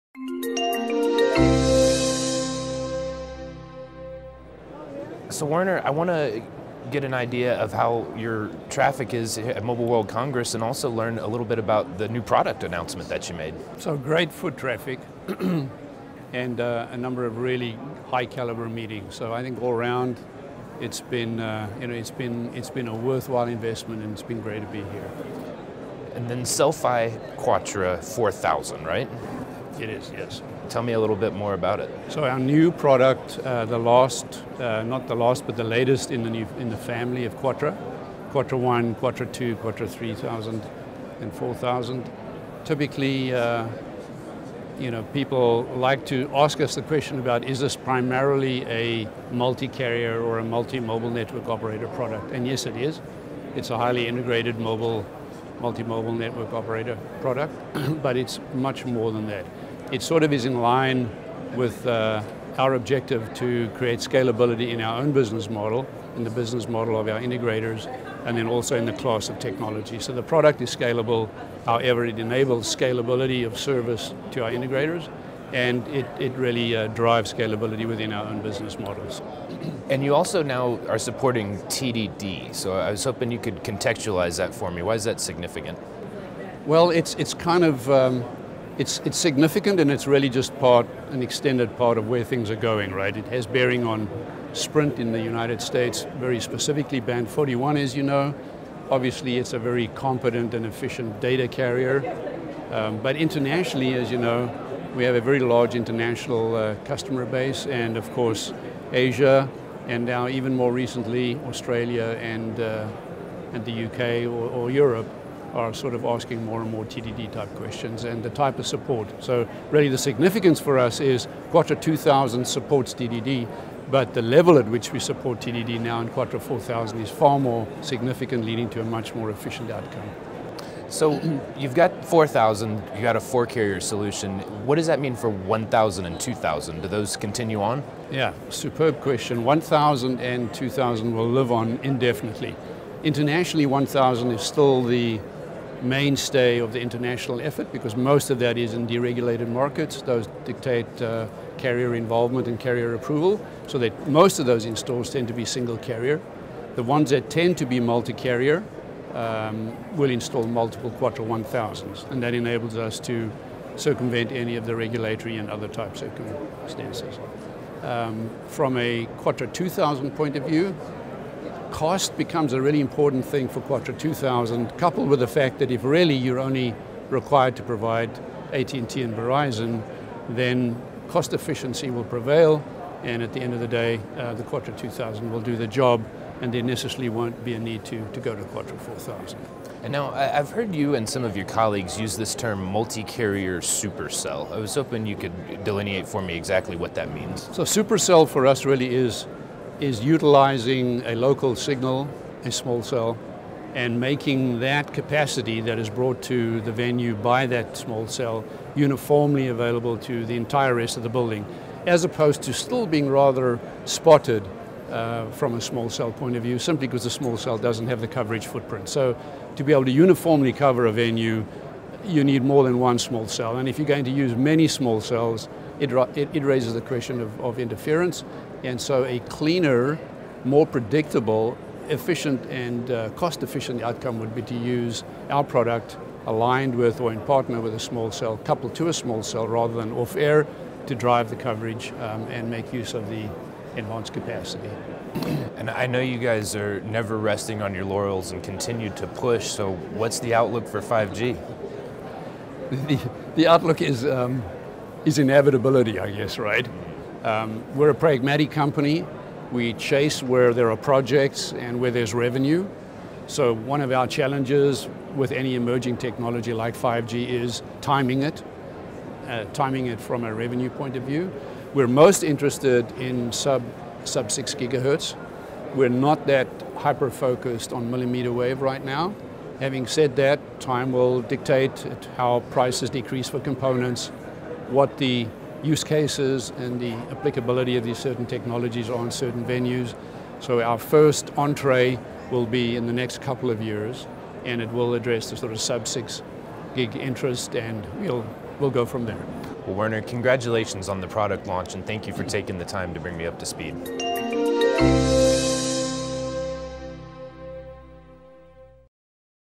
In-Building Communication at MWC19: Cel-Fi QUATRA 4000 builds on success
Nextivity continues to build on their enterprise platform with the introduction of the Cel-Fi QUATRA 4000.  Nextivity innovated in the DAS and Booster space by leveraging their award winning Intelliboost Chipset with the All-Digital RFoE / PoE architecture.  The Cel-Fi QUATRA 1000 and 2000 are also available, with a highly scalable architecture.  The new products are for use on AT&T, Verizon, Sprint, and T-Mobile networks.  The Cel-Fi QUATRA can be integrated with carrier small cells, creating a better distribution via the Cel-Fi QUATRA SuperCell.